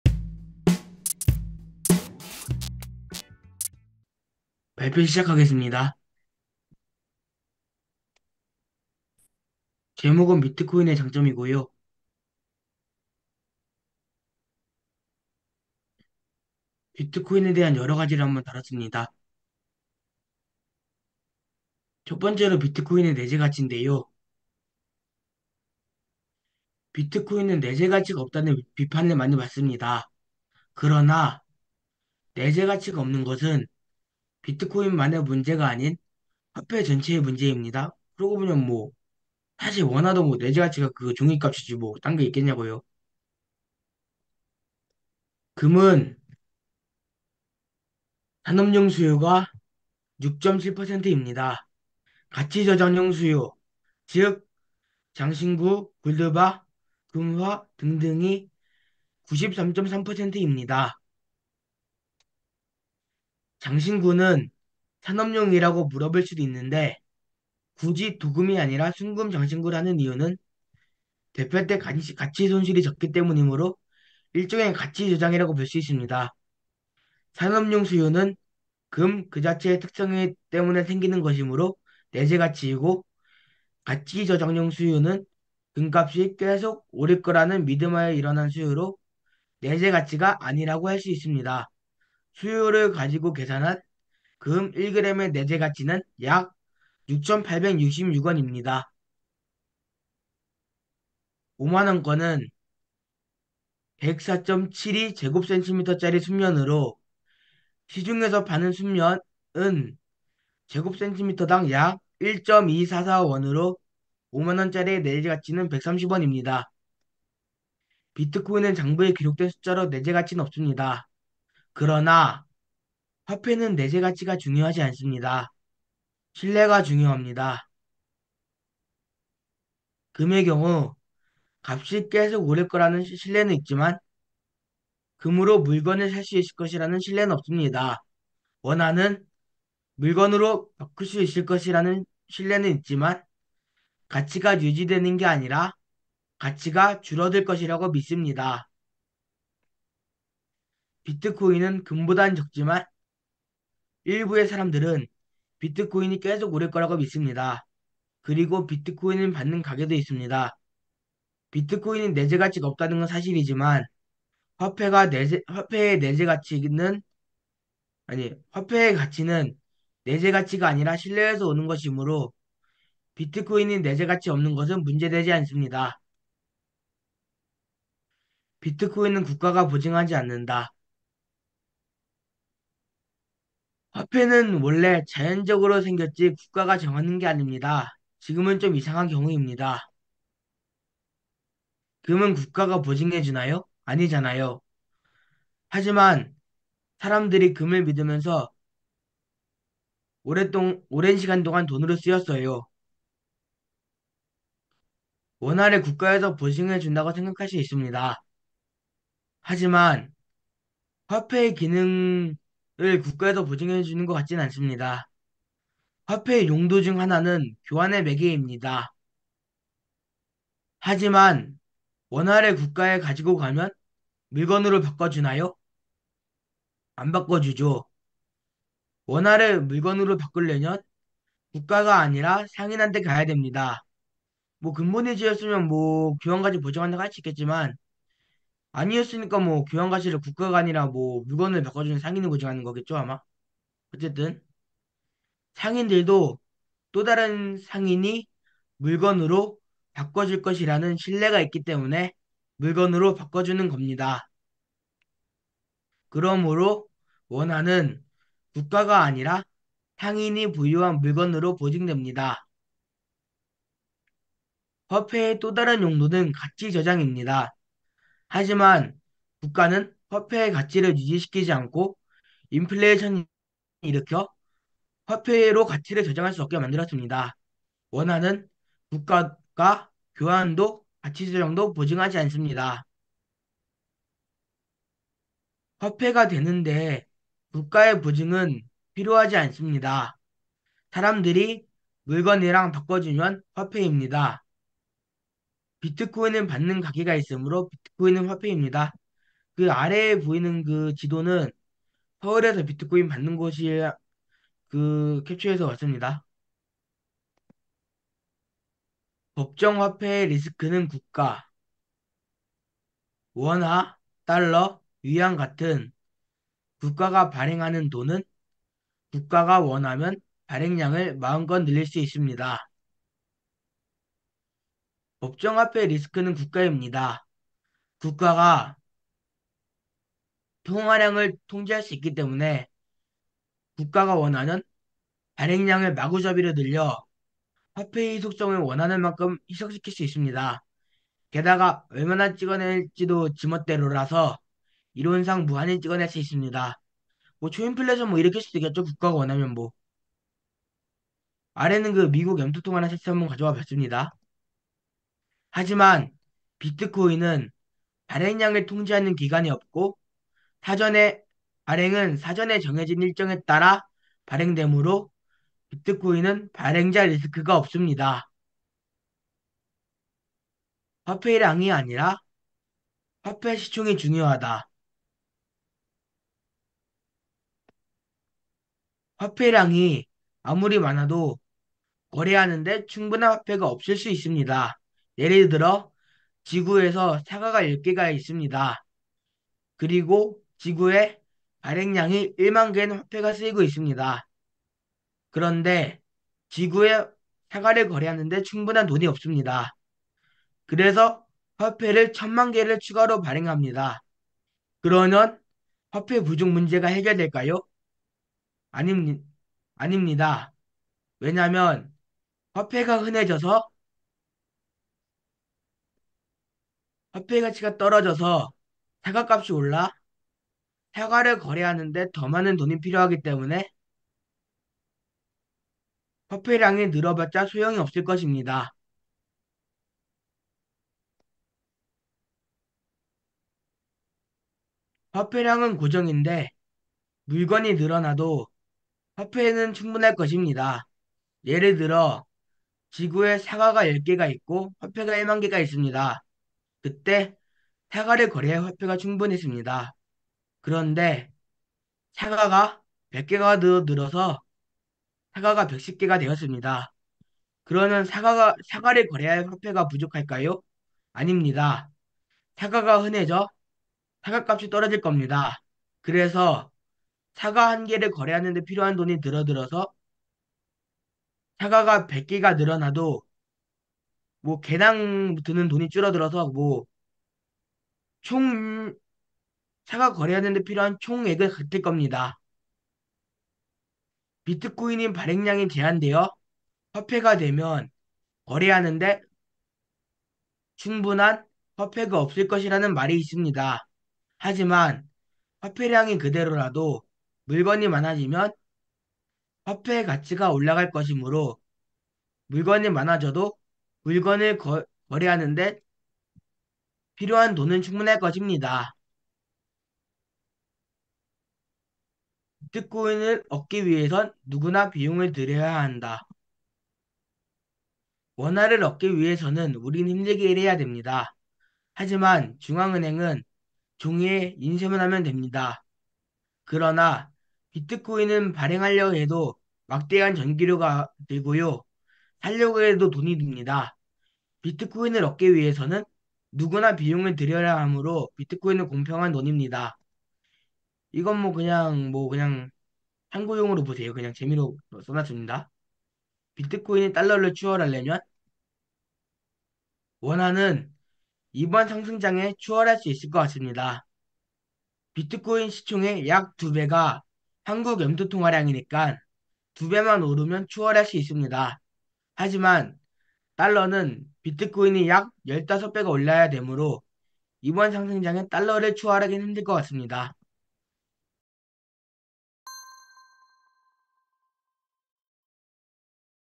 [블파스] 비트코인에 대한 견해 | 하태성
- 비트코인의 장점
- 비트코인의 내재가치
- 화폐에 대한 국가의 보증
- 화폐량이 아닌 화폐 시총의 중요성

블파스 - 블록체인 파헤치는 스터디 17기